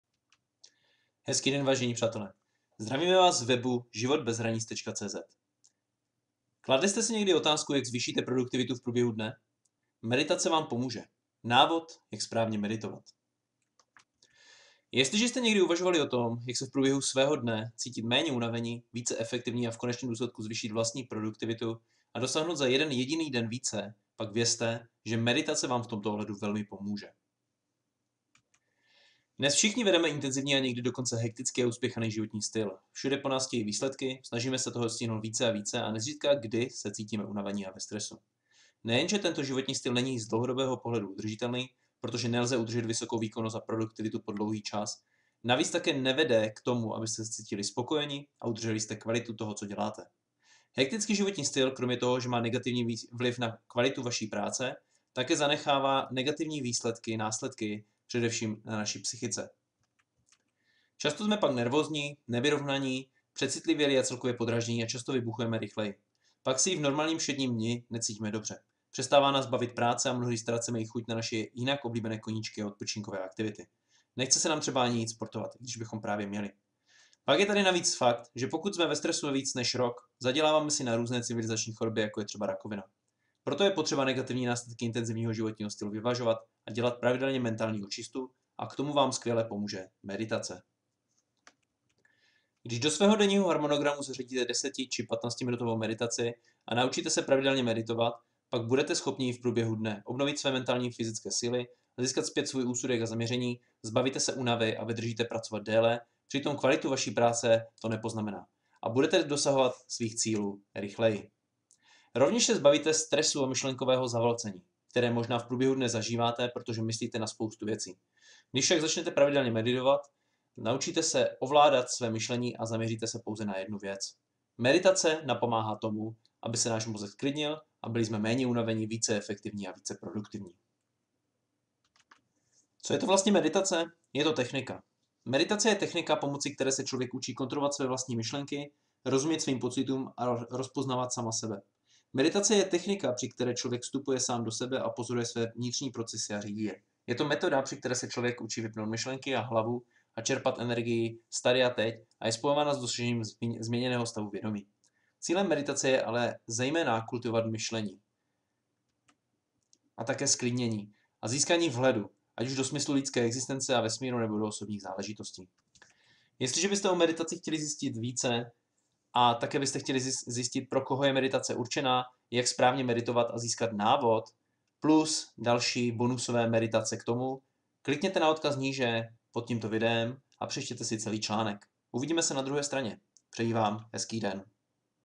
Jak zvýšíte produktivitu v průběhu dne? Meditace Vám pomůže + Návod Jak Správně Meditovat...
Jestliže jste někdy uvažovali o tom, jak se v průběhu svého dne cítit méně unavení, více efektivní a v konečném důsledku zvýšit vlastní produktivitu a dosáhnout za jeden jediný den více, pak vězte, že meditace Vám v tomto ohledu velmi pomůže...

nes všichni vedeme intenzivní a někdy dokonce hektický a uspěchaný životní styl...

Všude po nás chtějí výsledky,
snažíme se toho stihnout více a více a
nezřídka kdy se cítíme unavení a ve stresu...

Nejenže tento životní styl není z dlouhodobého pohledu udržitelný, protože nelze udržet vysokou výkonnost a produktivitu po dlouhý čas...

Navíc také nevede k tomu, abyste se cítili spokojeni a udrželi jste kvalitu toho, co děláte.

Hektický životní styl - kromě toho, že má negativní vliv na kvalitu naší práce, také zanechává negativní následky především na naší psychice.

Často jsme pak nervózní, nevyrovnaní, přecitlivělí a celkově podráždění a často vybuchujeme rychleji.

Pak se i v normálním všedním dni necítíme dobře, přestává nás bavit práce a mnohdy ztrácíme i chuť na naše jinak oblíbené koníčky a odpočínkové aktivity.

Nechce se nám třeba ani jít sportovat, i když bychom právě měli.

Pak je tady navíc fakt, že pokud jsme ve stresu více než rok, zaděláváme si na různé civilizační choroby jako třeba rakovina.

Proto je potřeba negativní následky intenzivního životního stylu vyvažovat a dělat pravidelně mentální očistu a k tomu Vám skvěle pomůže meditace.